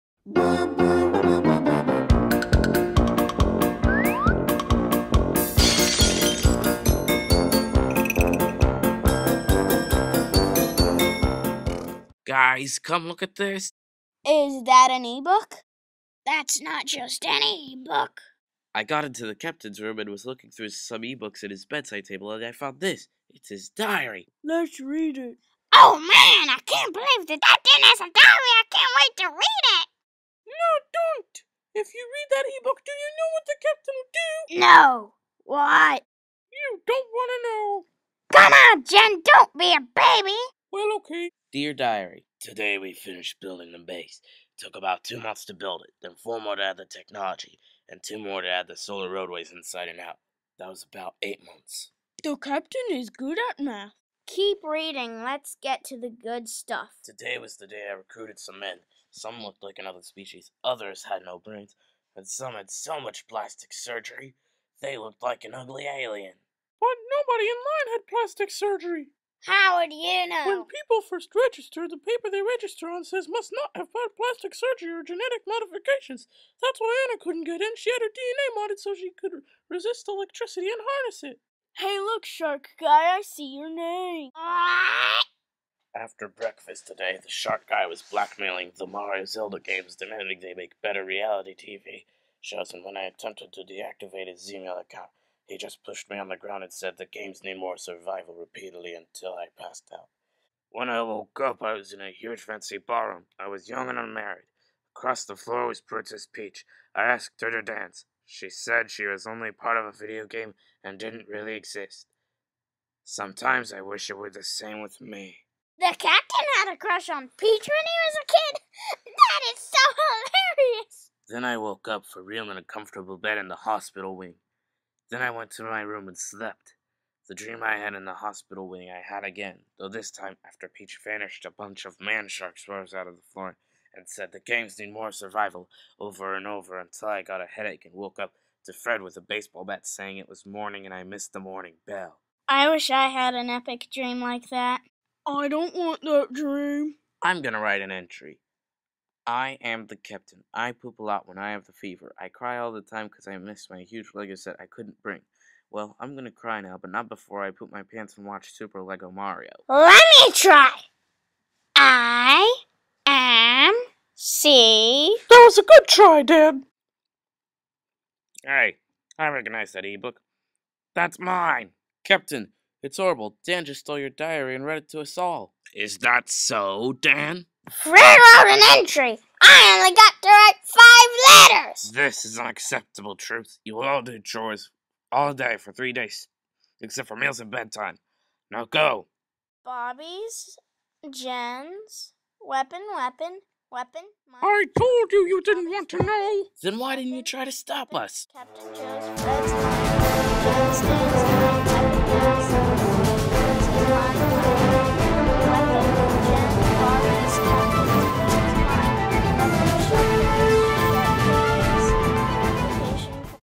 The Dangers of having a Diary
The Captain left his Diary unguarded, and while he was sleeping, Fred stole it and read it to everyone in the Base! What will happen to them?? This is a one time episode, unless you guys want more!